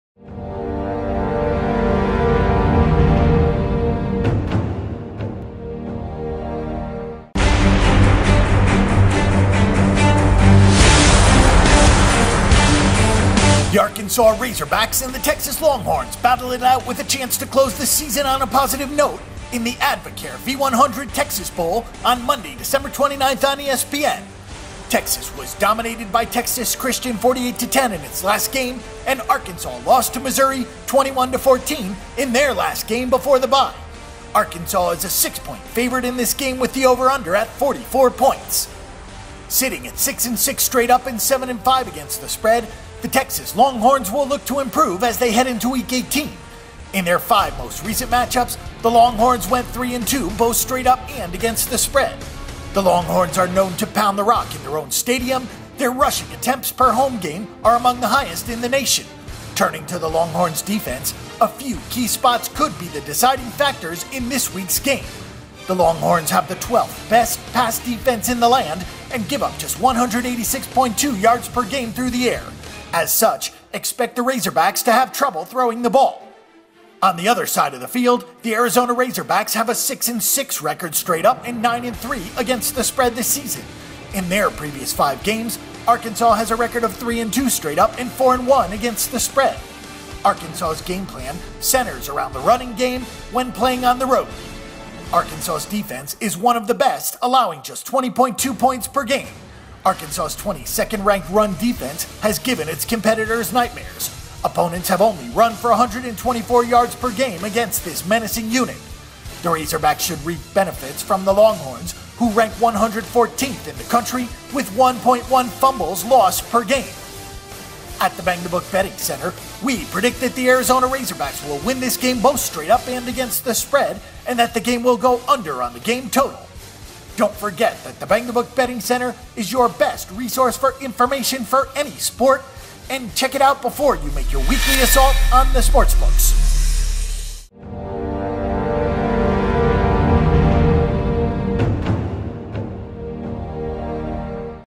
Texas Bowl Arkansas Razorbacks vs Texas Longhorns | Odds, Picks and Analysis
Full Arkansas Razorbacks vs Texas Longhorns Advocare V100 Texas Bowl pick;

The Arkansas Razorbacks (6-6) and the Texas Longhorns (6-6) battle it out with a chance to close out the season on a positive note in the Advocare V100 Texas Bowl on Monday December 29. The game will begin at 9:00 p.m. ET and can be seen on ESPN.